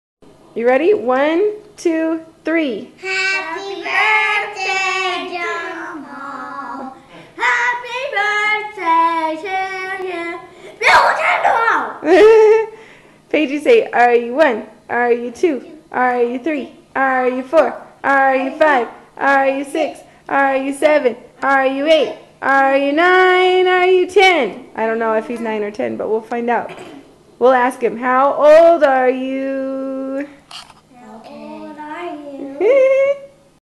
Happy Birthday John Paul
We missed you on your birthday and we wanted you to know we're thinking about you.